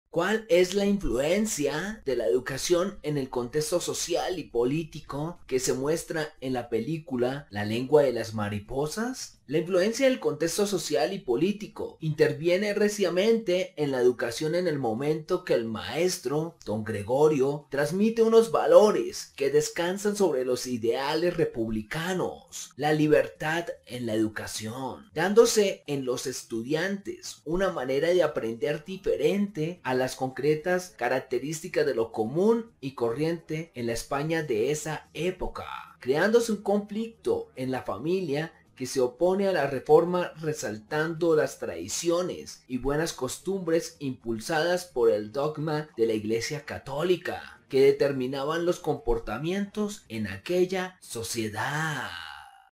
# Metarquía / La Lengua de la Mariposas-Influencia de la educación en el contexto social y político.
En este vídeo explicaré acerca de  La Lengua de la Mariposas-Influencia de la educación en el contexto social y político eso va ayudar a sobrellevar lo que vivimos, aquello que tocan los extremos de las emociones y los sentimientos.